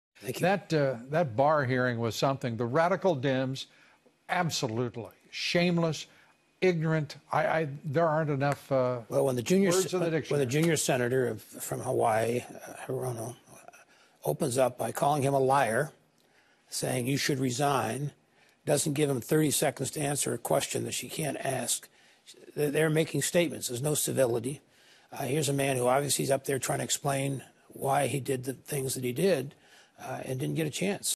Senator Hirono called Barr a liar and said he should resign with no chance for response
Sen. Hirono called Barr a liar & said he should resign. She didnt' give him 30 seconds to answer a question. The Dimms were making statements, there was no civility. Barr didn’t get a chance to even explain himself